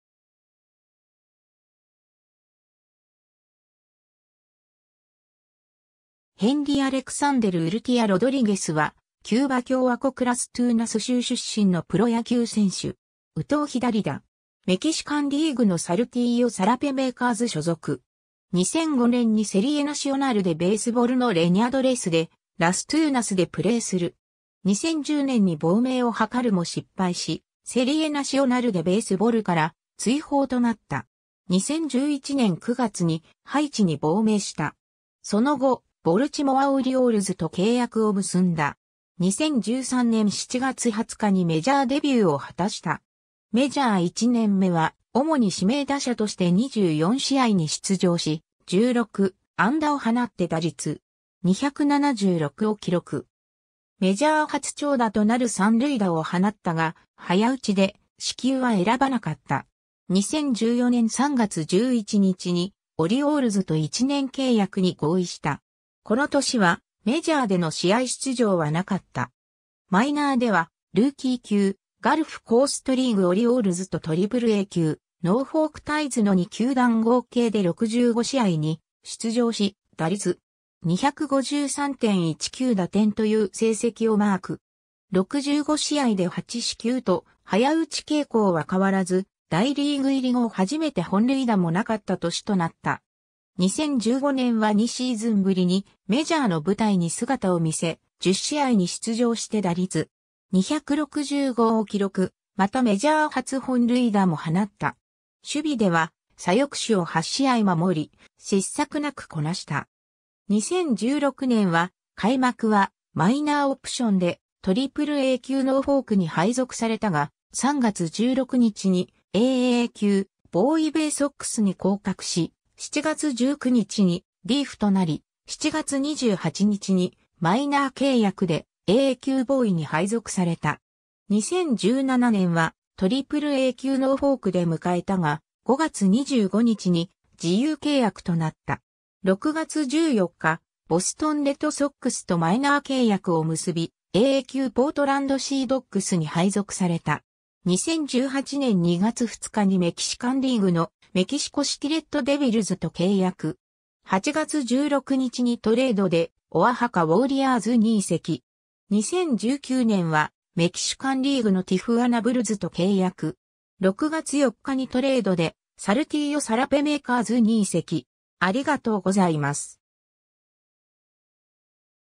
ヘンリー・ウルティア
レニャドレス・デ・ラス・トゥーナスの選手
メキシコシティ・レッドデビルズの選手
オアハカ・ウォーリアーズの選手
ティフアナ・ブルズの選手
サルティーヨ・サラペメーカーズの選手
ラス・トゥーナス州出身の人物
ヘンリー・アレクサンデル・ウルティア・ロドリゲス（Henry Alexander Urrutia Rodríguez, 1987年2月13日 - ）は、キューバ共和国ラス・トゥーナス州出身のプロ野球選手（外野手）。
右投左打。
メキシカンリーグのサルティーヨ・サラペメーカーズ所属。
 2005年にセリエ・ナシオナル・デ・ベイスボルのレニャドレス・デ・ラス・トゥーナスでプレーする。
 2010年に亡命を計るも失敗し、セリエ・ナシオナル・デ・ベイスボルから追放となった。
 2011年9月にハイチに亡命した。
その後、ボルチモア・オリオールズと契約を結んだ。
 2013年7月20日にメジャーデビューを果たした。
メジャー1年目は、主に指名打者として24試合に出場し、16安打を放って打率.276を記録。
メジャー初長打となる三塁打を放ったが、早打ちで四球は選ばなかった。
 2014年3月11日にオリオールズと1年契約に合意した。
この年は、メジャーでの試合出場はなかった。
マイナーでは、ルーキー級ガルフ・コーストリーグ・オリオールズとAAA級ノーフォーク・タイズの2球団合計で65試合に出場し、打率.253・19打点という成績をマーク。
65試合で8四球と早打ち傾向は変わらず、大リーグ入り後初めて本塁打もなかった年となった。
 2015年は2シーズンぶりにメジャーの舞台に姿を見せ、10試合に出場して打率.265を記録、またメジャー初本塁打も放った。
守備では左翼手を8試合守り、失策なくこなした。
 2016年は、開幕はマイナー・オプションでAAA級ノーフォークに配属されたが、3月16日にAA級ボウイ・ベイソックスに降格し、7月19日にDFAとなり、7月28日にマイナー契約でAA級ボウイに配属された。
 2017年は、AAA級ノーフォークで迎えたが、5月25日に自由契約となった。
6月14日、ボストン・レッドソックスとマイナー契約を結び、AA級ポートランド・シードッグスに配属された。
 2018年2月2日にメキシカンリーグのメキシコシティ・レッドデビルズと契約。
8月16日にトレードでオアハカ・ウォーリアーズに移籍。
 2019年はメキシカンリーグのティフアナ・ブルズと契約。
6月4日にトレードでサルティーヨ・サラペメーカーズに移籍。